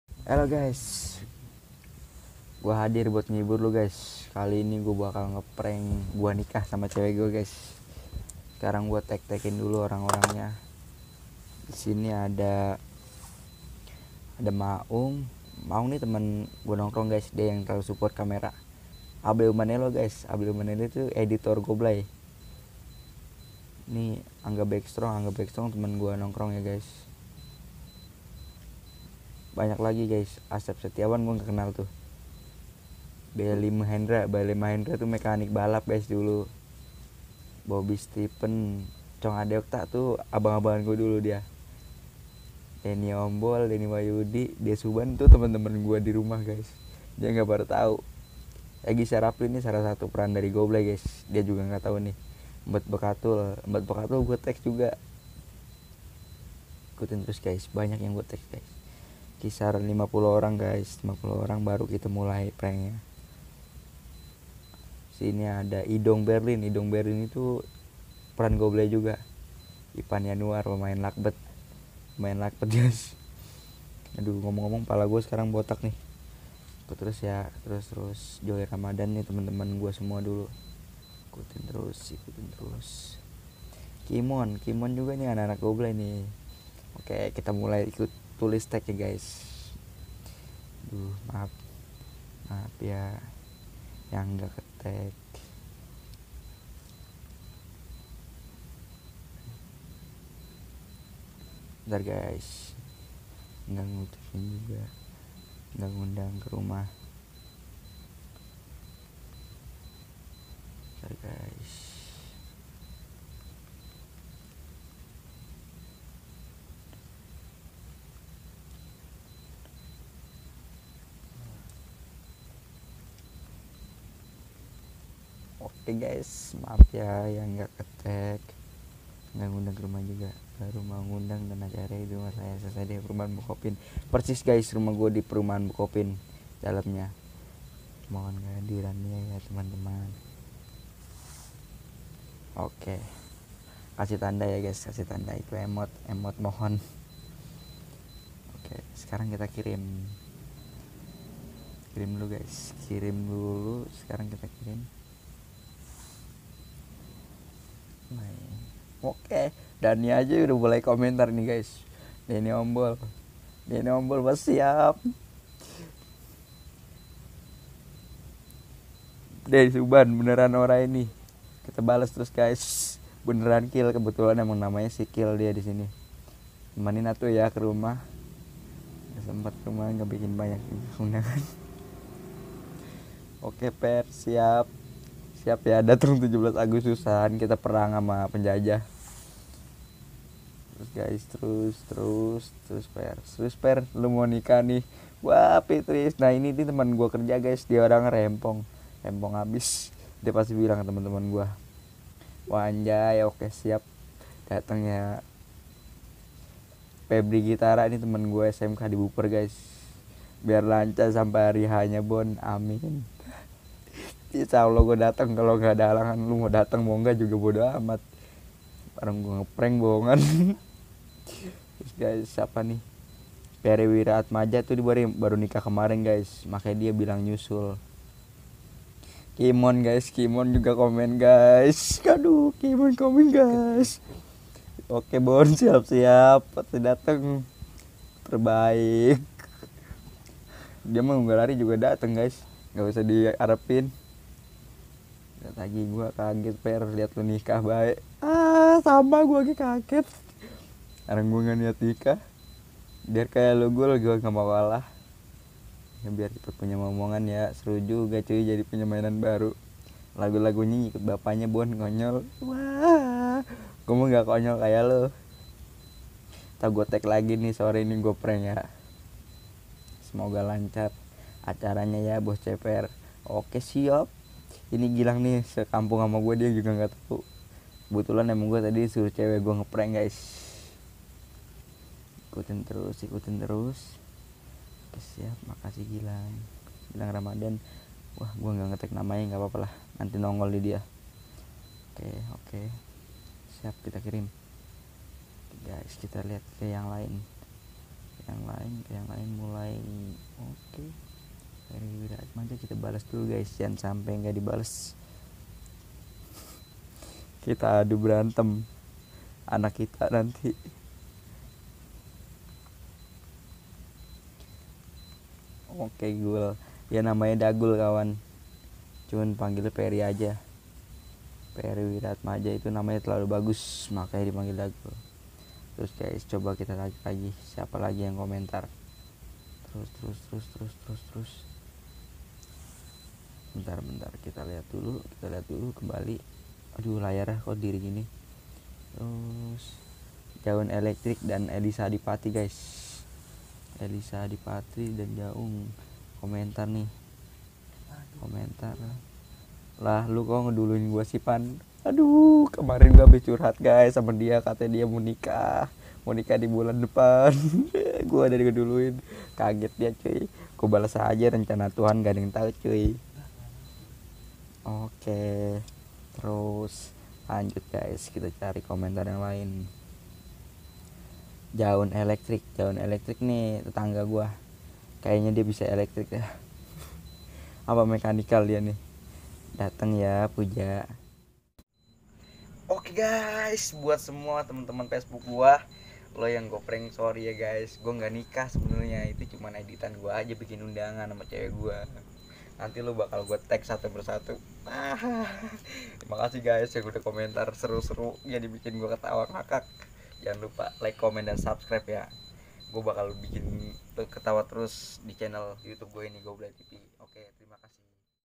Ngerjain GUA NIKAH SAMA CEWE GUA😁TEMEN"PADA MAU DATENG.....
Lu pasti ngakak guys.sampe 100 orang lebih yg komen😁😂